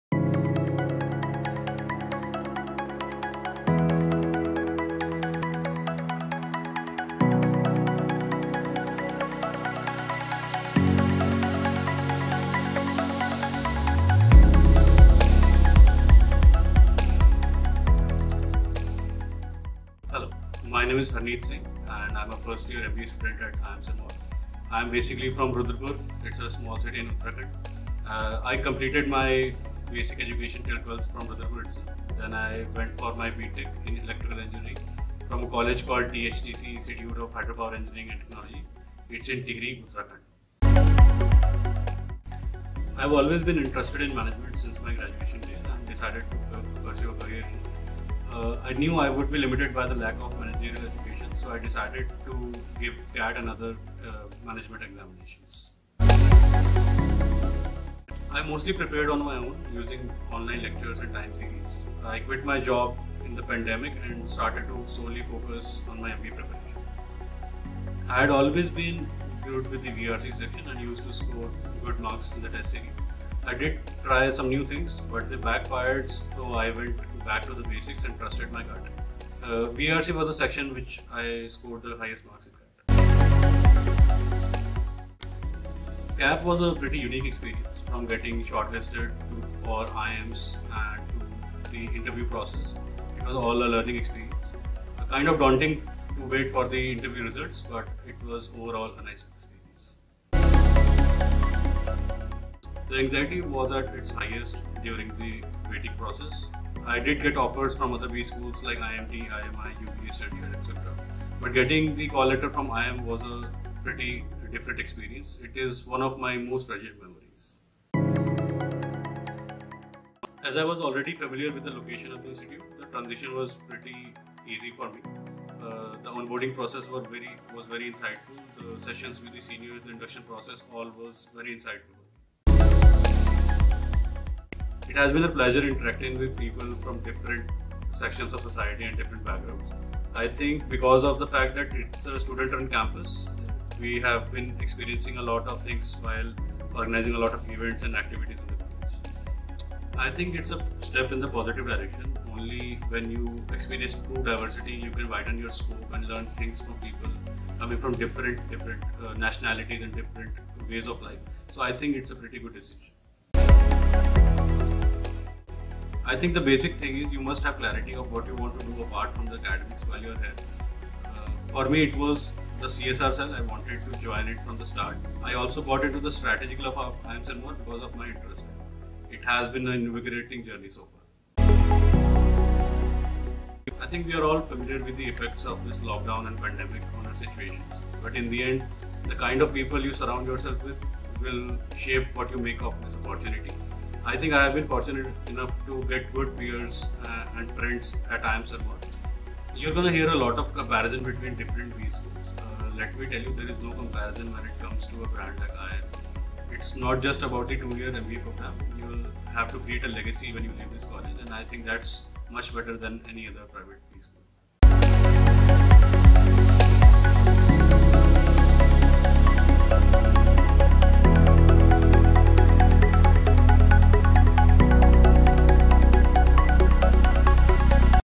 Fresher's Talk | Season 2 | Episode 4 | IIM Sirmaur
For the fourth episode, we have Harneet Singh, a Bachelor of Technology  in Electrical Engineering, who’ll enlighten us on his CAT to IIM journey.

Hoping that you will watch and enjoy our humble effort at bringing you some interesting perspectives on the CAT adventure, Team Horizon wishes you the best of luck in your journey and looks forward to meeting some of you on campus next year.